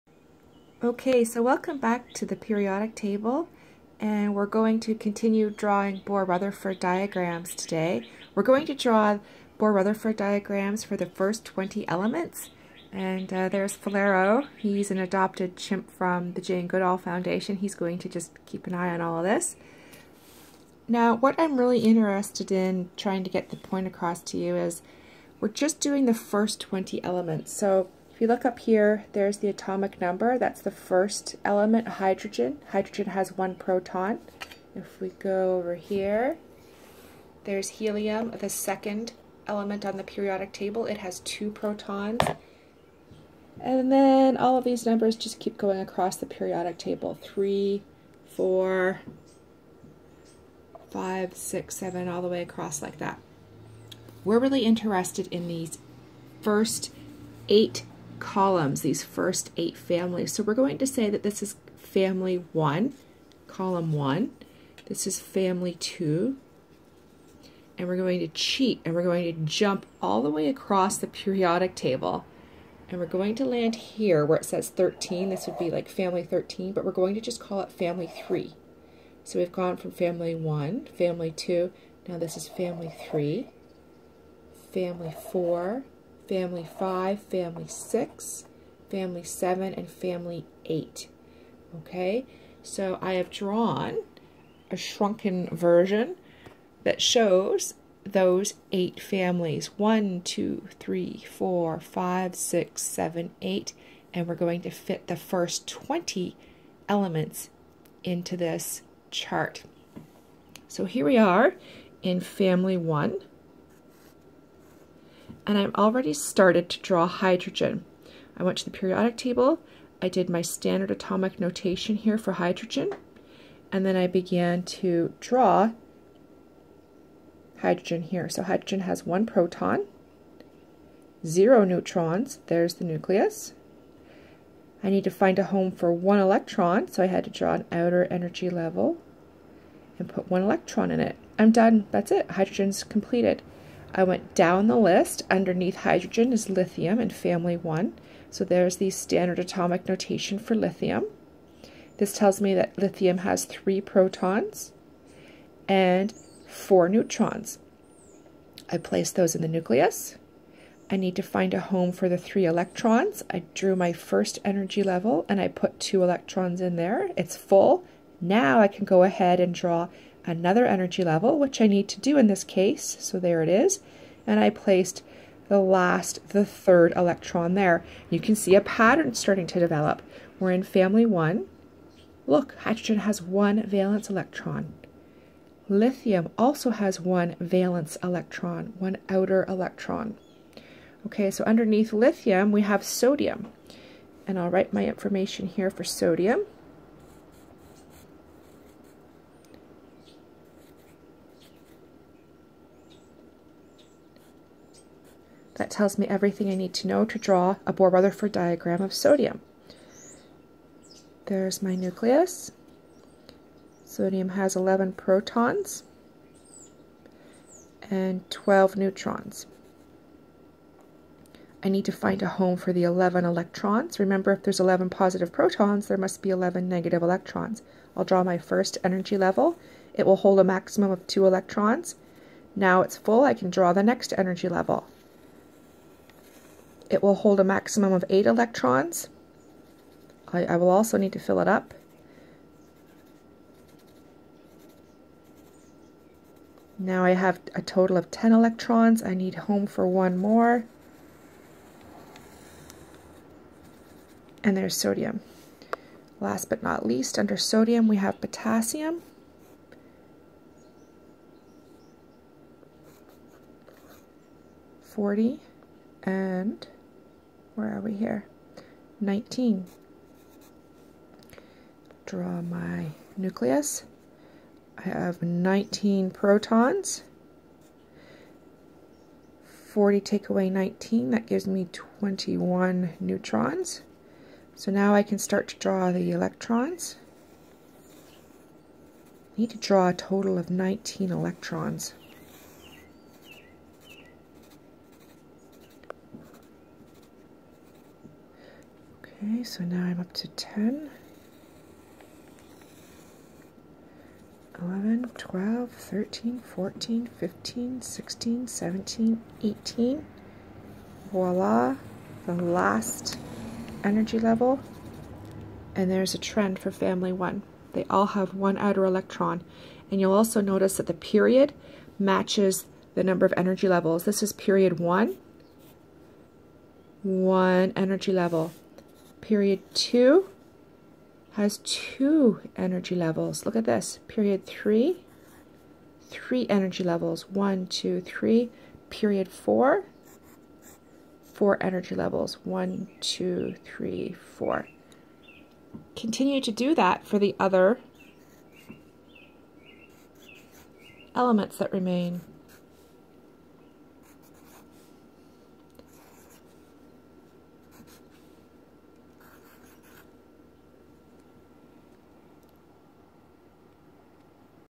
2) Bohr Rutherford First 20 Elements
We produced a "shrunken" periodic table to show the first 20 elements across 8 Families.

Family 1 elements all have 1 valence electeon.
Family 2 elements all have 2 valence electrons.
The pattern continues across all 8 Families.

The Period (row) that the element is in also corresponds with the number of energy levels (shells).
Period 1 elements have 1 energy level.
Period 2 elements have 2 energy levels.
Period 3 elements have 3 energy levels.
Period 4 elements have 4 energy levels.